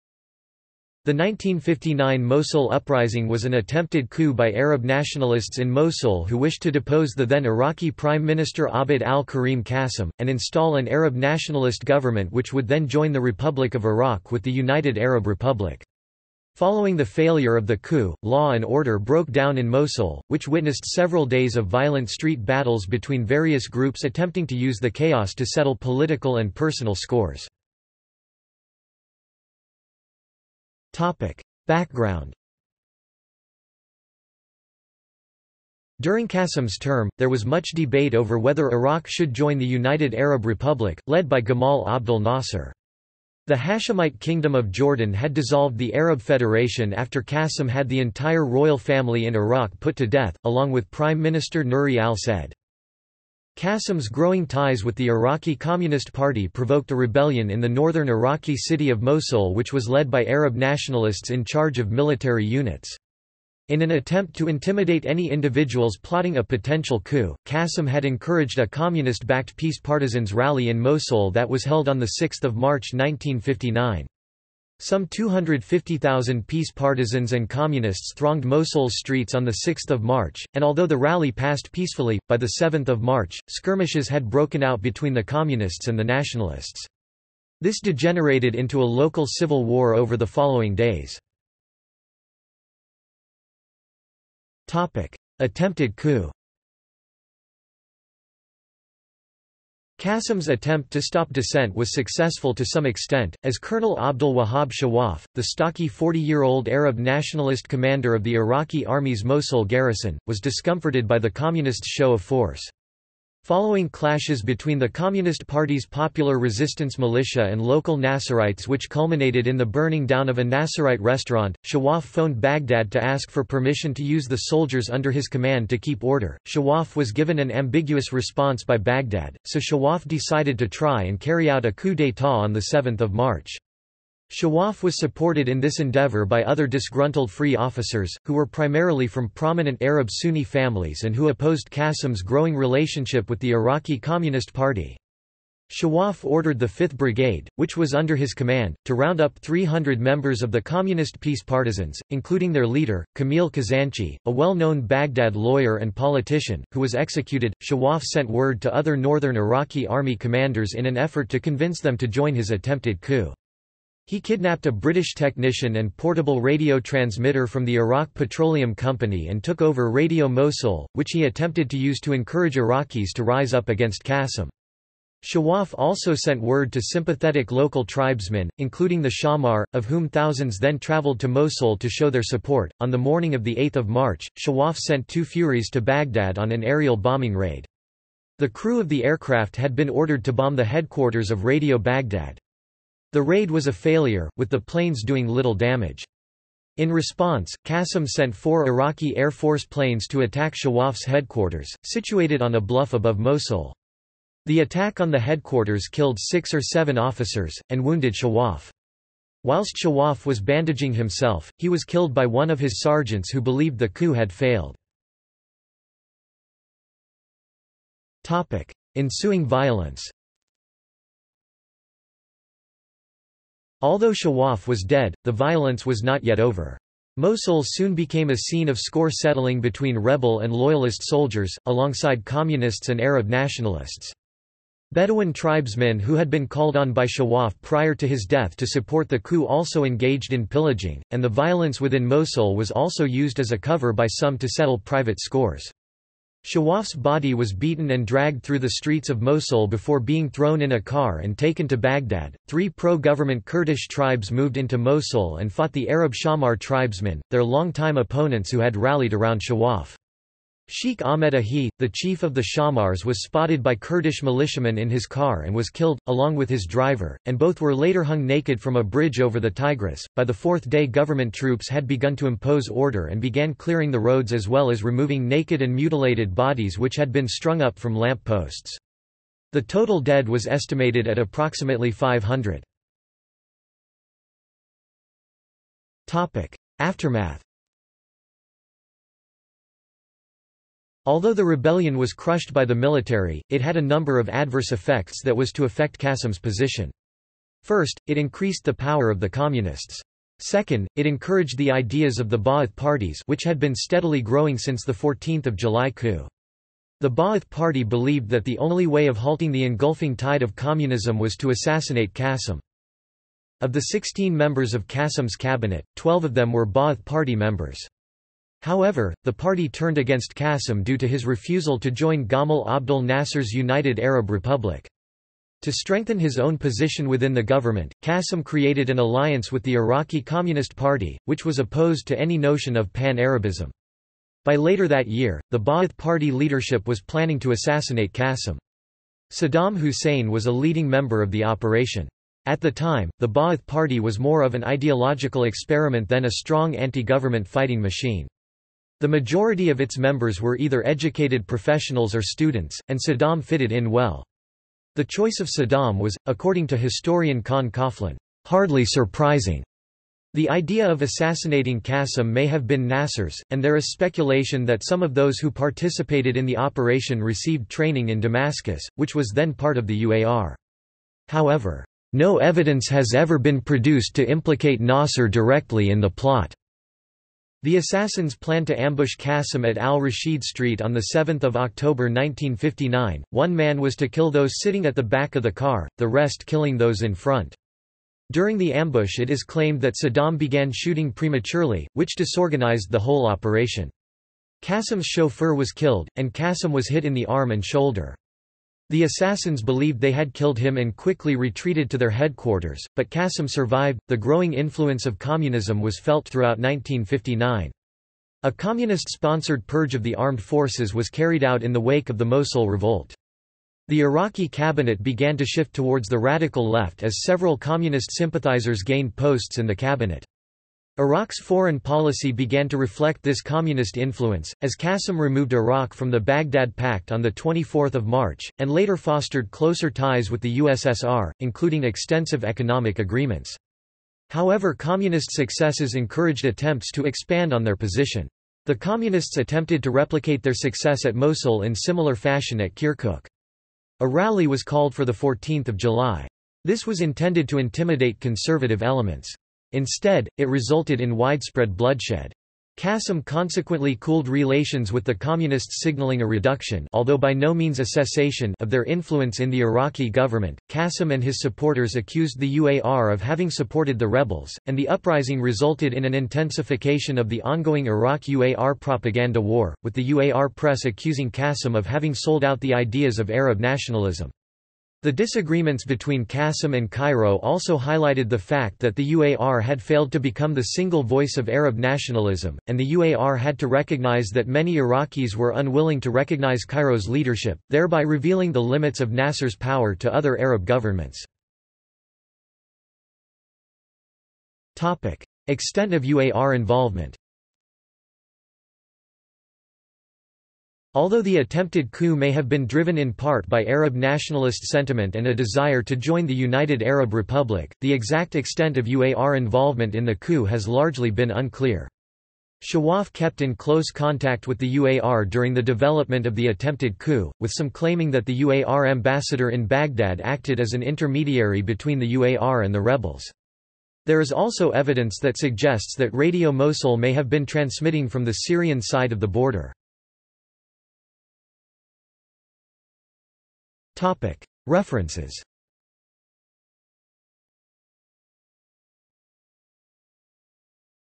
1959 Mosul uprising | Wikipedia audio article
This is an audio version of the Wikipedia Article:
1959 Mosul uprising

- Socrates

SUMMARY
The 1959 Mosul Uprising was an attempted coup by Arab nationalists in Mosul who wished to depose the then Iraqi Prime Minister Abd al-Karim Qasim, and install an Arab nationalist government which would then join the Republic of Iraq with the United Arab Republic. Following the failure of the coup, law and order broke down in Mosul, which witnessed several days of violent street battles between various groups attempting to use the chaos to settle political and personal scores.